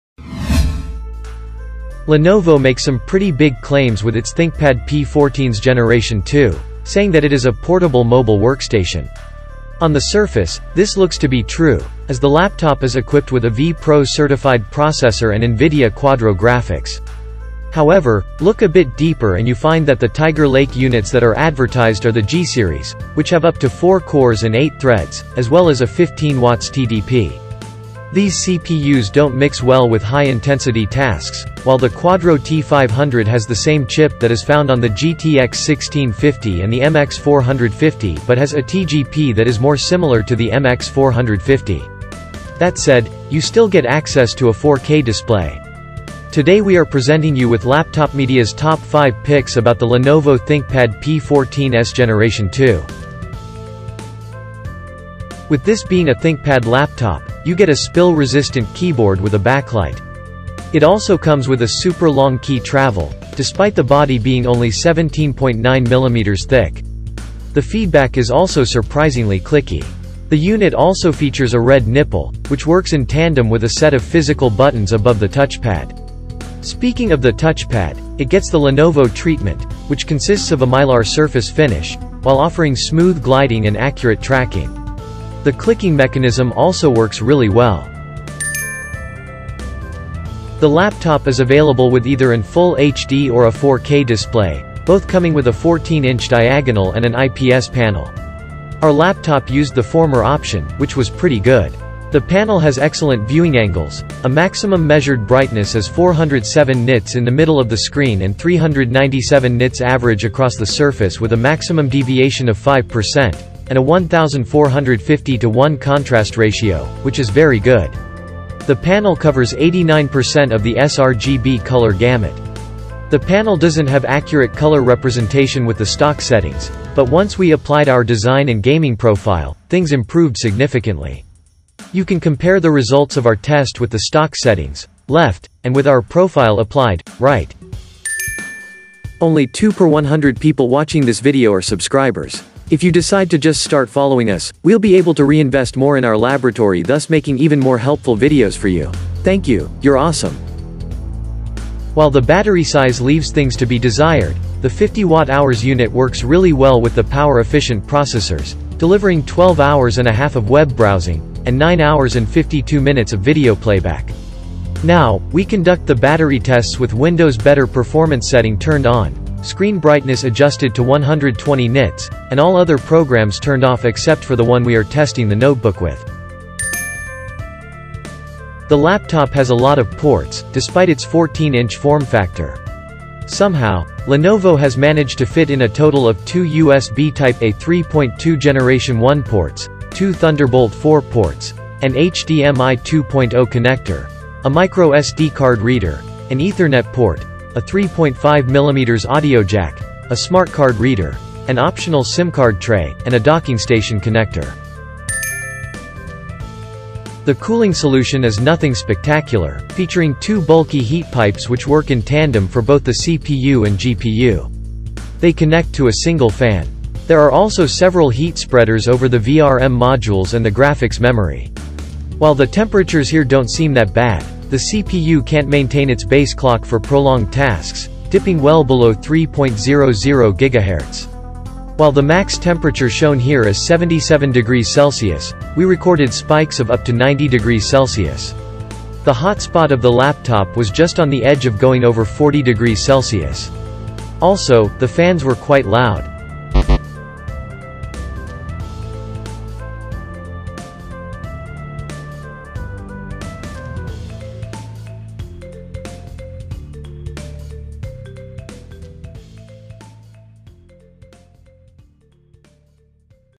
✅ ❌ Top 5 reasons to BUY or NOT to buy the Lenovo ThinkPad P14s Gen 2
✅  LaptopMedia.com

Top 5 reasons to BUY or NOT to buy the ASUS VivoBook S14 S435
➡️  CONTENTS:
[0:00] Intro
[0:54] Keyboard
[1:29] Display
[2:31] Battery
[2:57] Ports
[3:26] Cooling